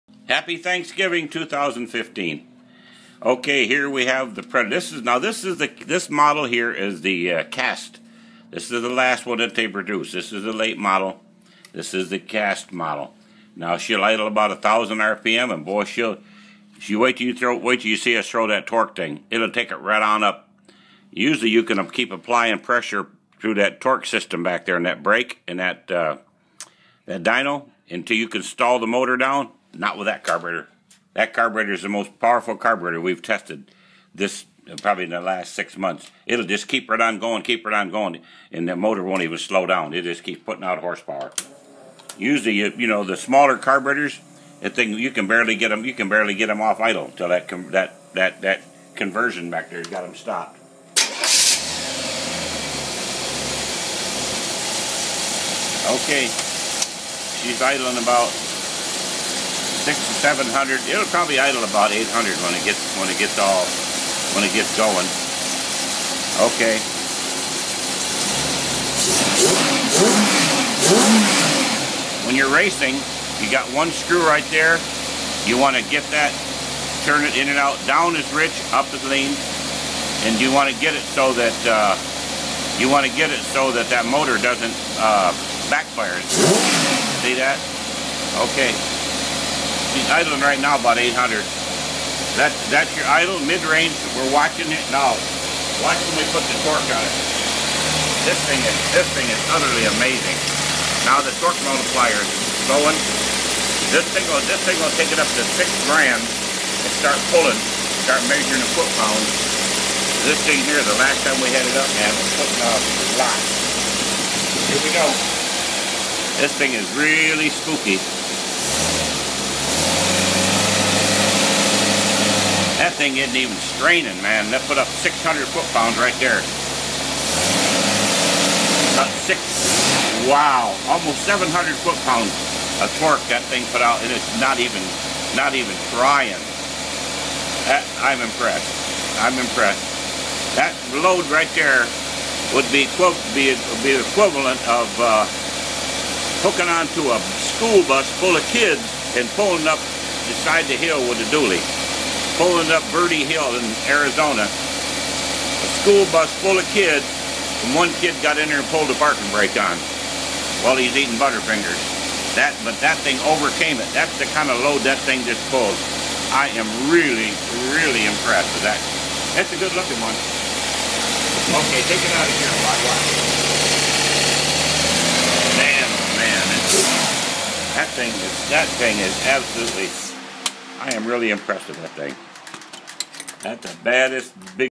Predator carb
This is a predator carb that is being tested on a running engine.